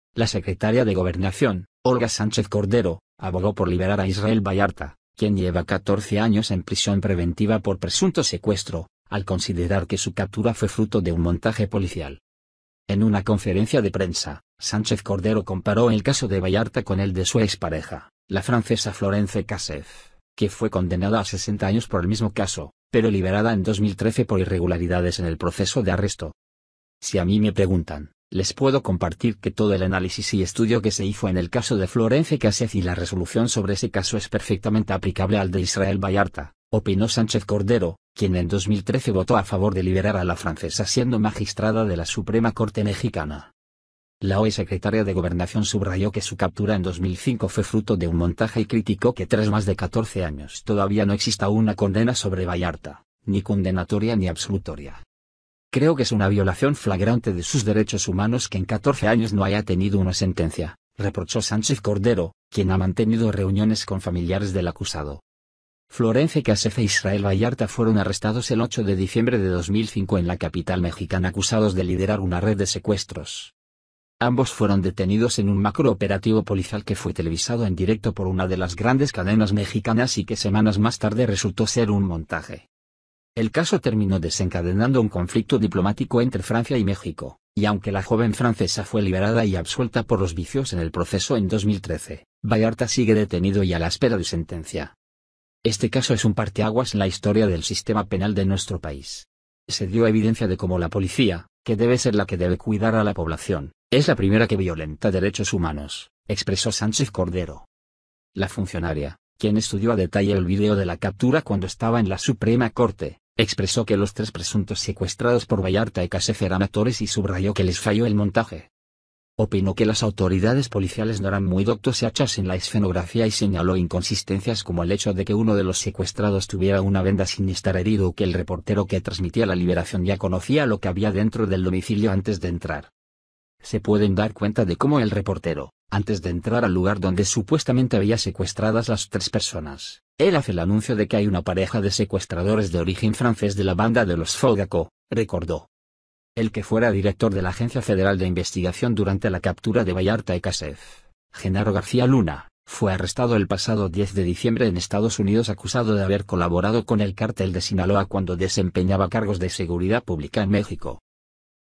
Sánchez Cordero aboga por liberar a Israel Vallarta expareja de Florence Cassez
La secretaria de Gobernación, Olga Sánchez Cordero, abogó por liberar a Israel Vallarta, quien lleva 14 años en prisión preventiva por presunto secuestro, al considerar que su captura fue fruto de un “montaje” policial.

En una conferencia de prensa, Sánchez Cordero comparó el caso de Vallarta con el de su expareja, la francesa Florence Cassez, que fue condenada a 60 años por el mismo caso, pero liberada en 2013 por irregularidades en el proceso de arresto.

“Si a mí me preguntan, les puedo compartir que todo el análisis y estudio que se hizo en el caso de Florence Cassez y la resolución sobre ese caso es perfectamente aplicable al de Israel Vallarta”, opinó Sánchez Cordero, quien en 2013 votó a favor de liberar a la francesa siendo magistrada de la Suprema Corte mexicana.

La hoy secretaria de Gobernación subrayó que su captura en 2005 fue fruto de un “montaje” y criticó que tras más de 14 años todavía no exista una condena sobre Vallarta, ni condenatoria ni absolutoria.

“Creo que es una violación flagrante de sus derechos humanos que en 14 años no haya tenido una sentencia”, reprochó Sánchez Cordero, quien ha mantenido reuniones con familiares del acusado.

Florence Cassez e Israel Vallarta fueron arrestados el 8 de diciembre de 2005 en la capital mexicana acusados de liderar una red de secuestros.

Ambos fueron detenidos en un macrooperativo policial que fue televisado en directo por una de las grandes cadenas mexicanas y que semanas más tarde resultó ser un montaje.

El caso terminó desencadenando un conflicto diplomático entre Francia y México, y aunque la joven francesa fue liberada y absuelta por los vicios en el proceso en 2013, Vallarta sigue detenido y a la espera de sentencia.

“Este caso es un parteaguas en la historia del sistema penal de nuestro país. Se dio evidencia de cómo la policía, que debe ser la que debe cuidar a la población, es la primera que violenta derechos humanos”, expresó Sánchez Cordero.

La funcionaria, quien estudió a detalle el vídeo de la captura cuando estaba en la Suprema Corte, expresó que los tres presuntos secuestrados por Vallarta y Cassez “eran actores” y subrayó que “les falló el montaje”.

Opinó que las autoridades policiales “no eran muy doctos y hachas en la escenografía” y señaló inconsistencias como el hecho de que uno de los secuestrados tuviera una venda sin estar herido o que el reportero que transmitía la liberación ya conocía lo que había dentro del domicilio antes de entrar.

“Se pueden dar cuenta de cómo el reportero, antes de entrar al lugar donde supuestamente había secuestradas las tres personas, él hace el anuncio de que hay una pareja de secuestradores de origen francés de la banda de Los Zodiaco”, recordó.

El que fuera director de la Agencia Federal de Investigación durante la captura de Vallarta y Cassez, Genaro García Luna, fue arrestado el pasado 10 de diciembre en Estados Unidos acusado de haber colaborado con el Cártel de Sinaloa cuando desempeñaba cargos de seguridad pública en México.